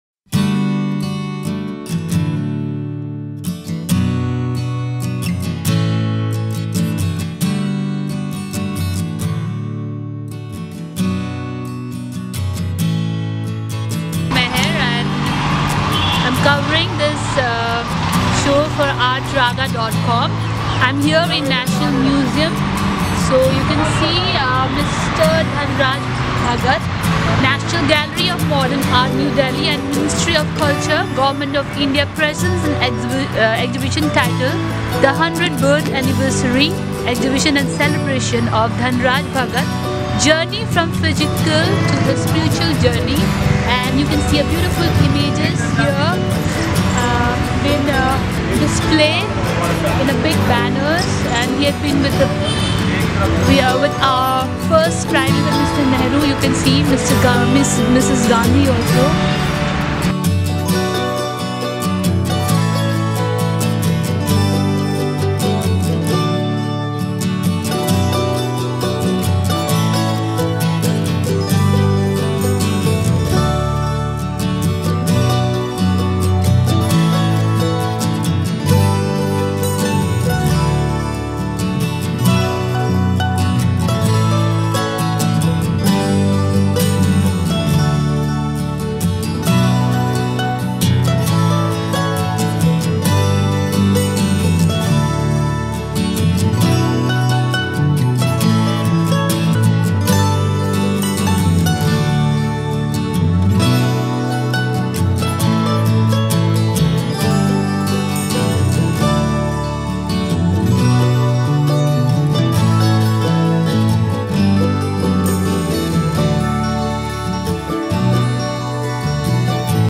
Dhanraj Bhagat
Dhanraj Bhagat an  Indain artist is regarded one of the most creative sculptors.The National Gallery of Modern Art, New Delhi showcases
exhibition featuring his work of eminent  artist Dhanraj Bhagat.